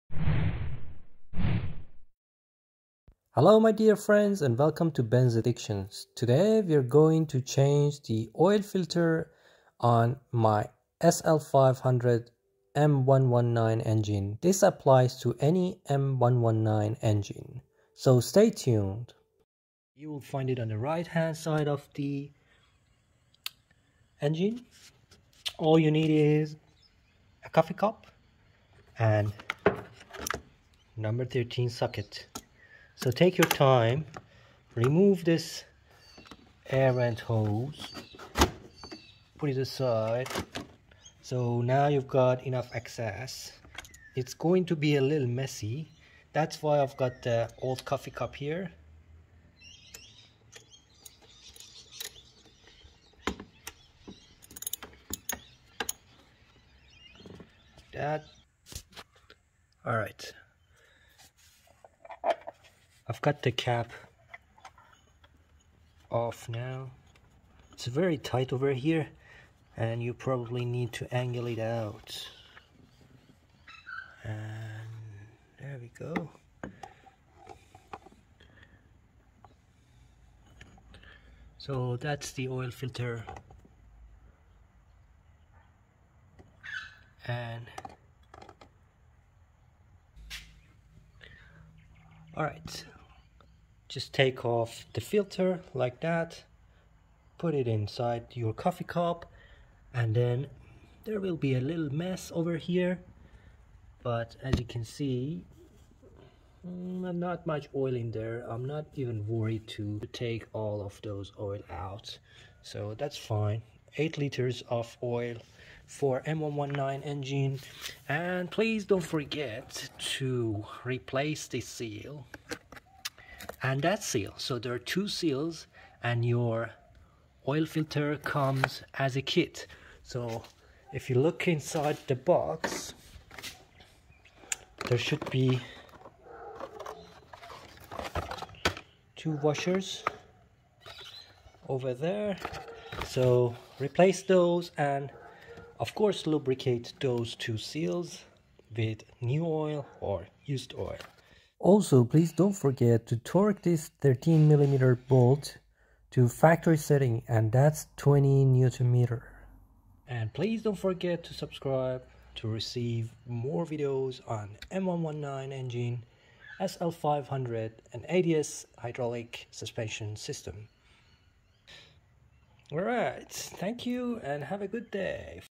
Oil Filter Change on R129 SL500 (M119 Engine)
Oil filter change on 1994 R129 SL500 M119. This process applies to any Mercedes with M119 engine.